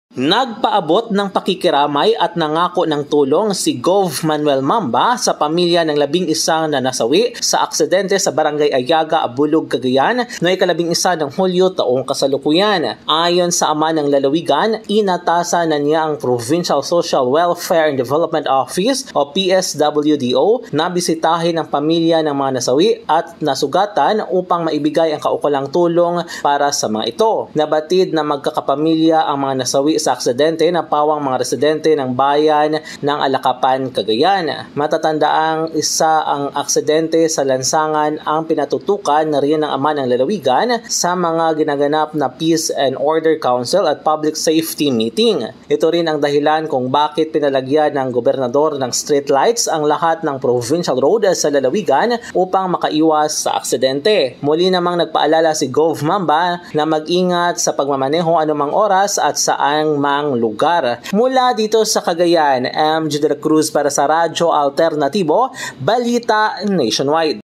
GOV. MAMBA, NAGPAABOT NG PAKIKIRAMAY AT TINIYAK ANG TULONG SA PAMILYA NG MGA NAAKSIDENTE SA CAGAYAN
GOV. MAMBA, NAGPAABOT NG PAKIKIRAMAY AT TINIYAK ANG TULONG SA PAMILYA NG MGA NAAKASIDENTE SA ABULUG, CAGAYAN

via | MJ Dela Cruz